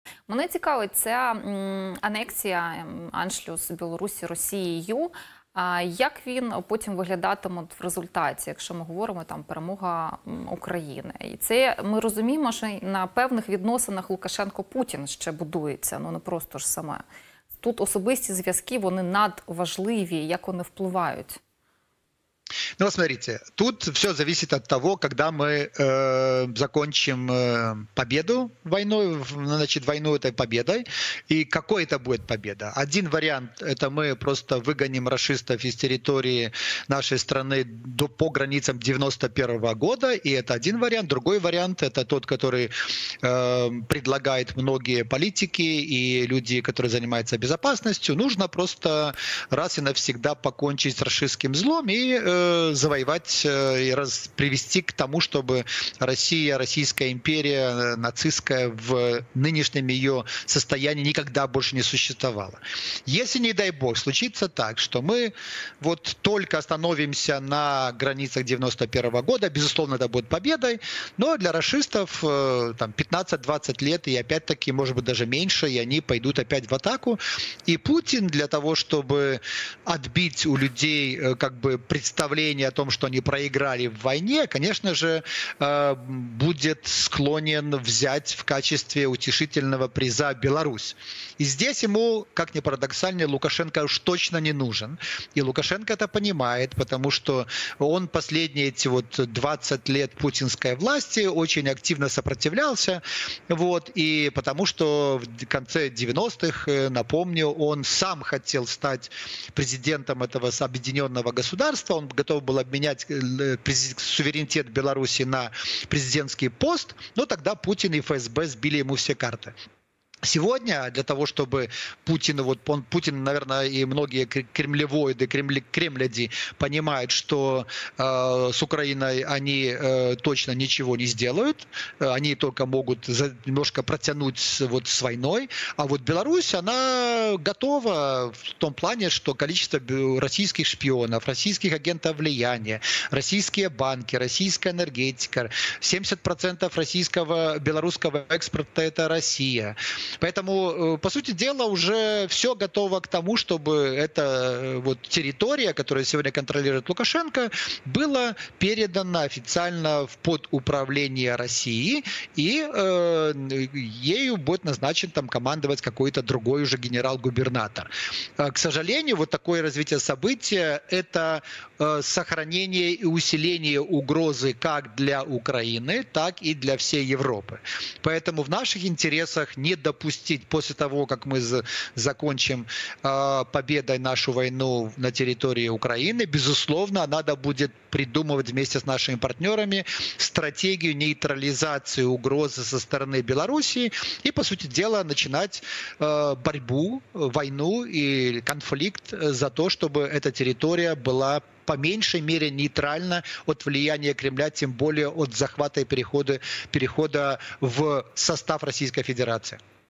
Путін може піти в АТАКУ і захопити БІЛОРУСЬ як втішний приз // Романчук
Спецефіри телеканалу «Апостроф TV» - Ми говоримо правду, а решту зроблять ЗСУ!  В етері: Ярослав Романчук, керівник з економічних реформ Офісу простих рішень та результатів, керівник Центру Мізеса, засновник білоруської школи ліберального сталого розвитку, кандидат у президенти Білорусі на виборах у 2010 р.